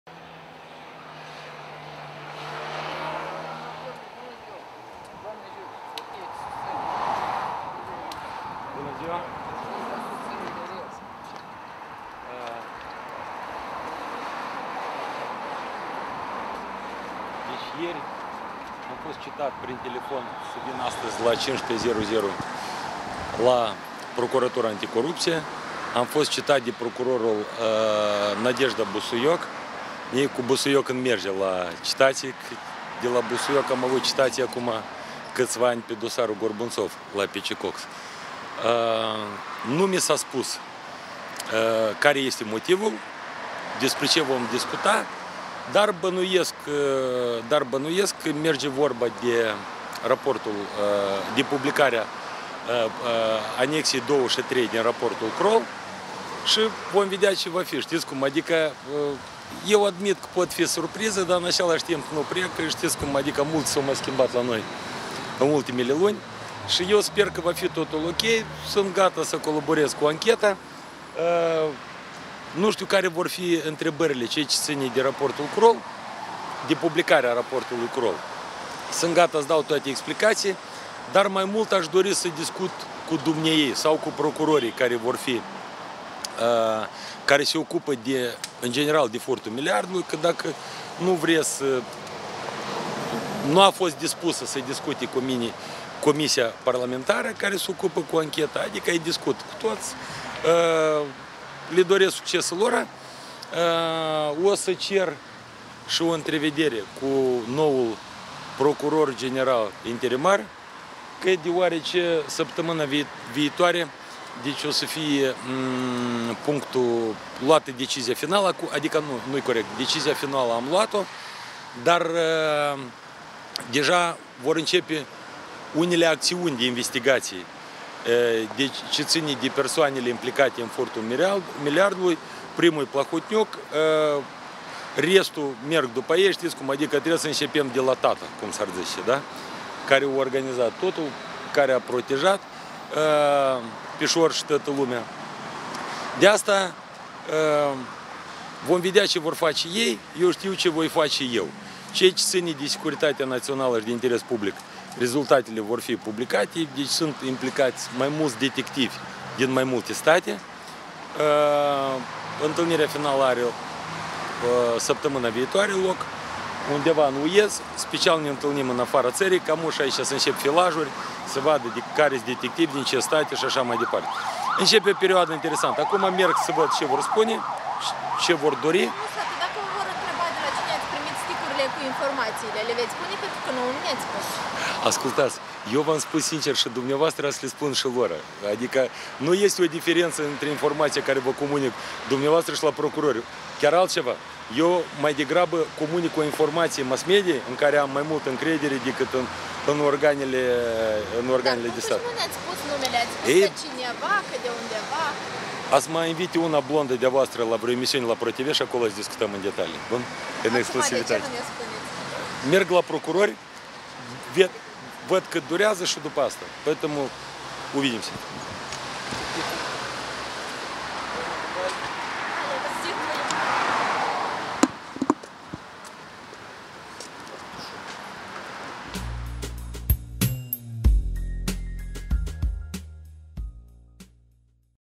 Ренато Усатый перед слушаниями в прокуратуре: мне есть, что рассказать и гражданам, и прокурорам
Ренато Усатый явился на слушания в Антикоррупционную прокуратуру, которые, вероятно, коснуться публикации доклада Kroll-2. Лидер «Нашей Партии» рассказал журналистам, что готов сотрудничать со следствием.